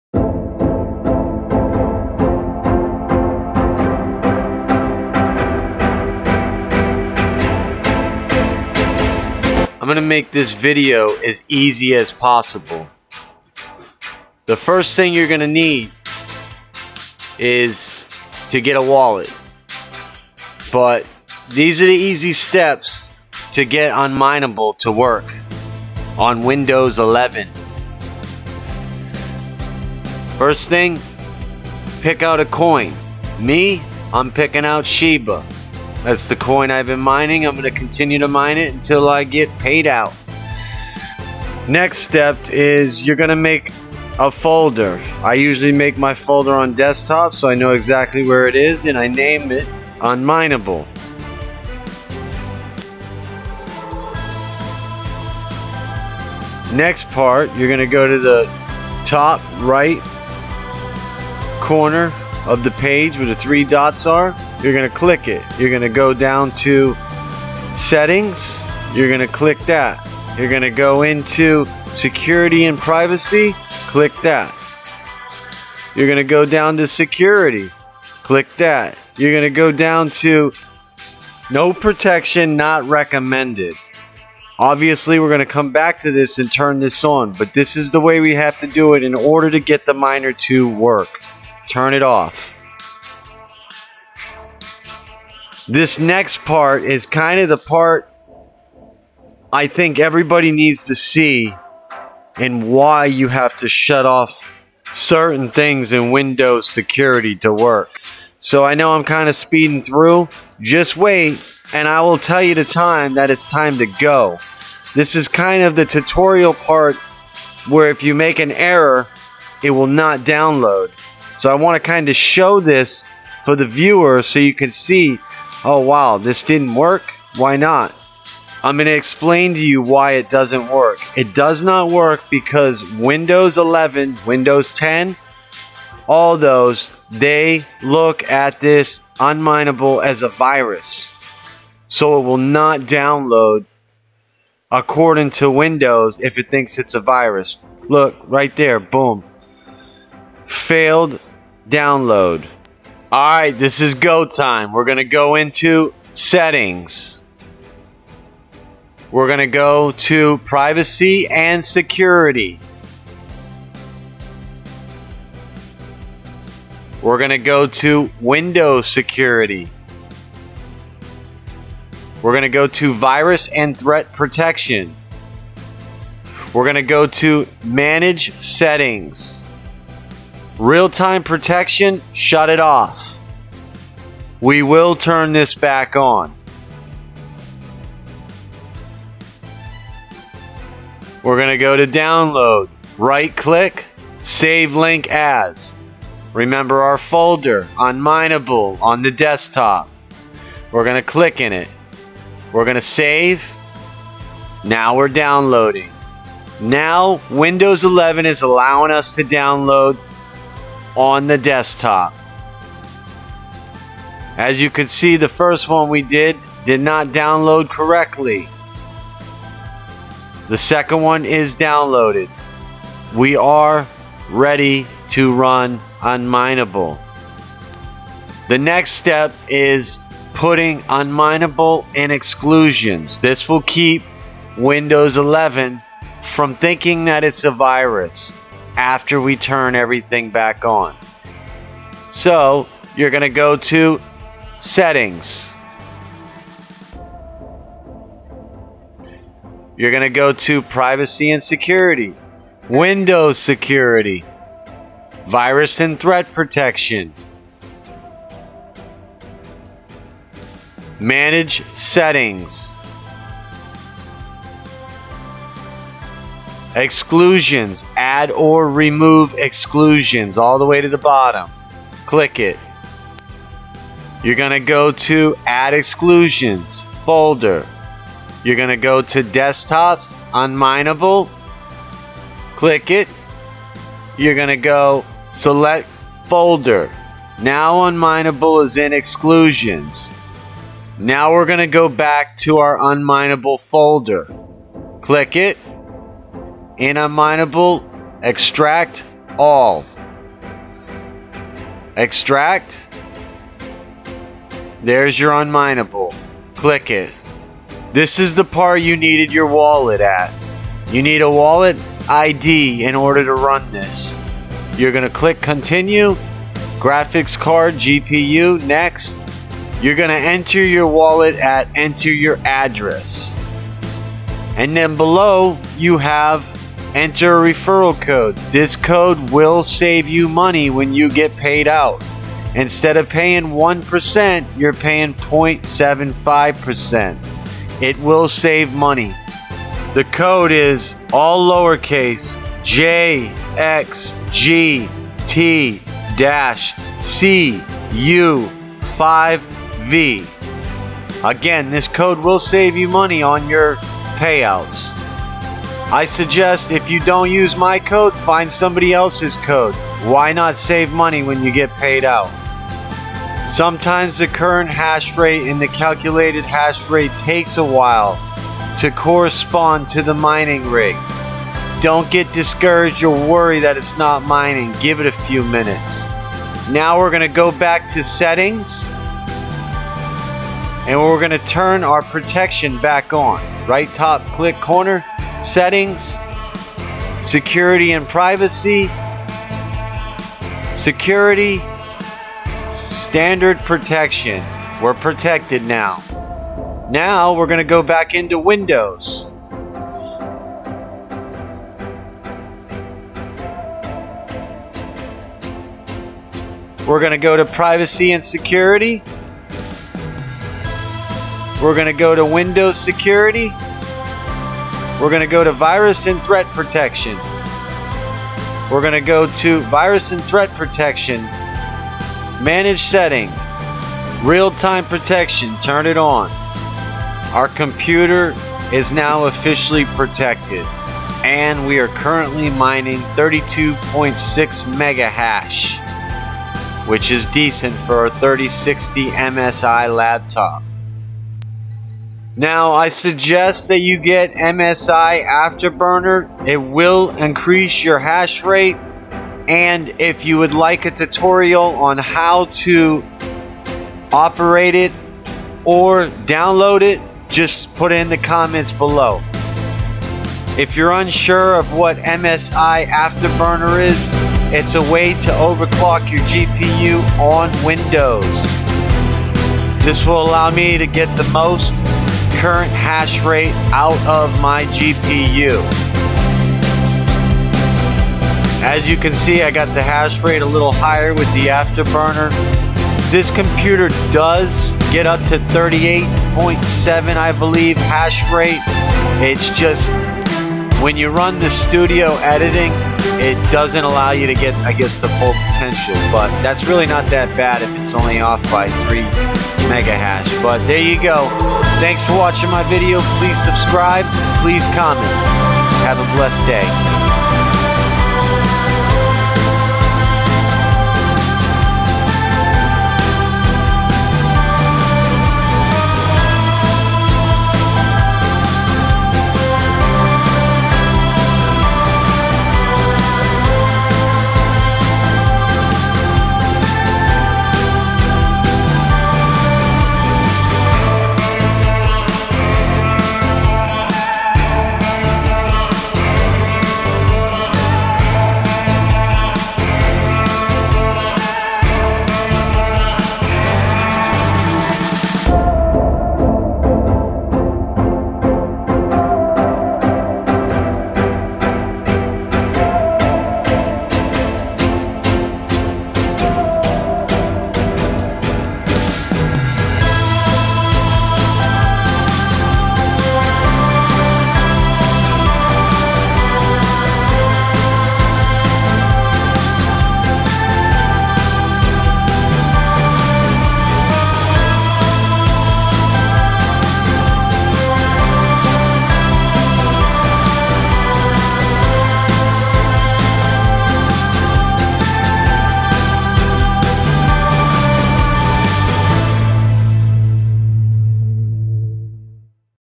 HOW TO INSTALL UNMINEABLE FOR WINDOWS 11
How to mine on unmineable

USE THIS CODE TO SAVE
jxgt-cu5v

0:00 intro
0:34 Pick out a coin to mine
1:05 security and privacy
1:39 download failed { skipping this part will speed up DOWNLOAD }
2:34 GO TIME window 11 settings
3:11 download success
4:01 Exclusions
4:59 unmineable folder extract
5:30 REFERAL CODE
6:25 security and privacy TURNED BACK ON
7:00 virus threat protection
7:35 intro on msi afterburner

CHECK OUT MY MINER FACEBOOK PG I WOULD LOVE TO SEE YOUR BUILDS PLEASE POST PLEASE COME CHAT WITH ME